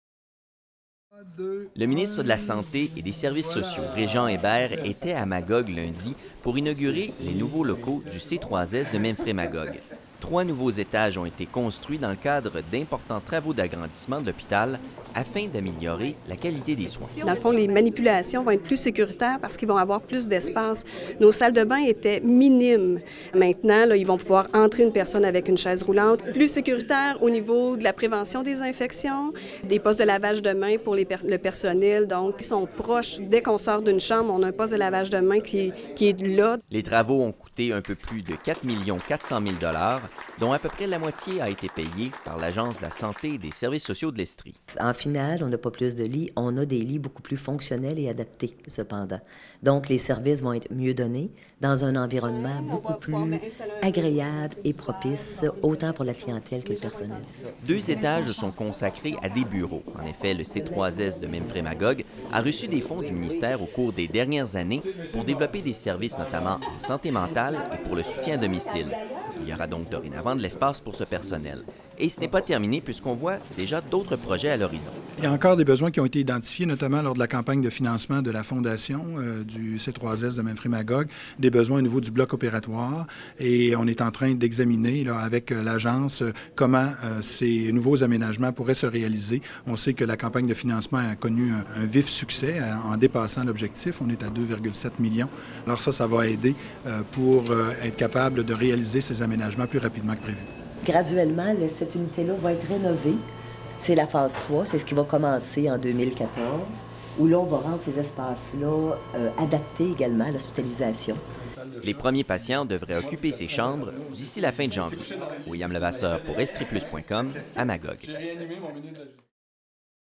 Le CSSS de Memphrémagog rehaussé de trois étages - EstriePlus.com
Le ministre de la Santé et des Services sociaux, ministre responsable des Aînés et ministre responsable de la région de l'Estrie, le docteur Réjean Hébert, a participé lundi à l'inauguration des nouveaux locaux du Centre de santé et de services sociaux (CSSS) de Memphrémagog.